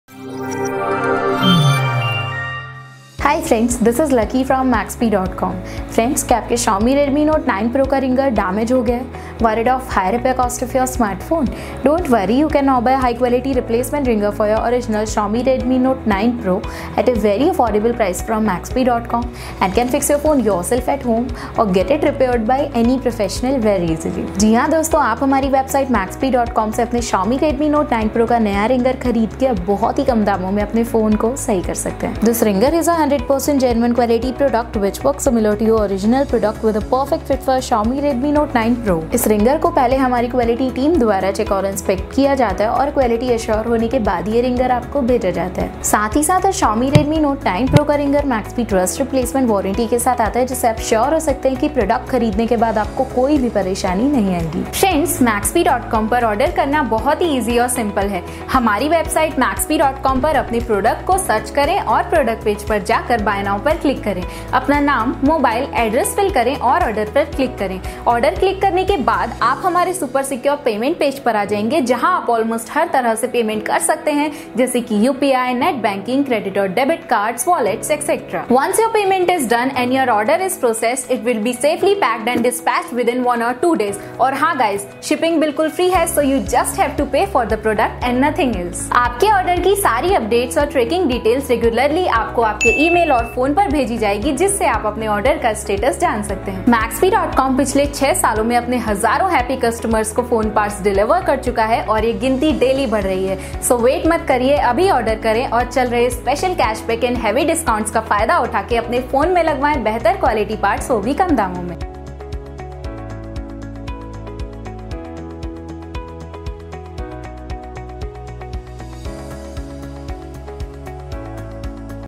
Buy Xiaomi Redmi Note 9 Pro Ringer, Free Delivery High Quality Best Price Maxbhi
Damaged Ringer in your Xiaomi Redmi Note 9 Pro? The replacement Ringer for Xiaomi Redmi Note 9 Pro comes with replacement warranty and the shipping is done in secured packing to make sure you get the product in perfect shape.